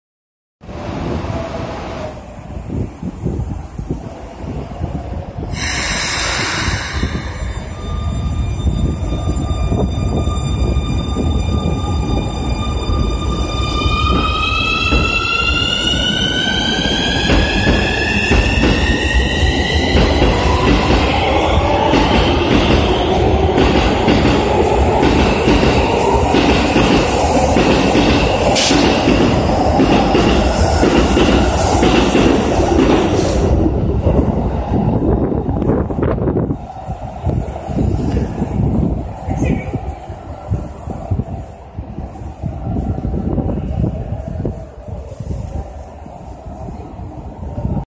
【”リラックマのイチゴお祝い号”の12両快特】京急新1000形(1065F+1401F) 京急蒲田駅発車
平成30年4月23日(月)撮影

1702A(80行路) 快特京急久里浜行き(1401Fは、金沢文庫で切り離し)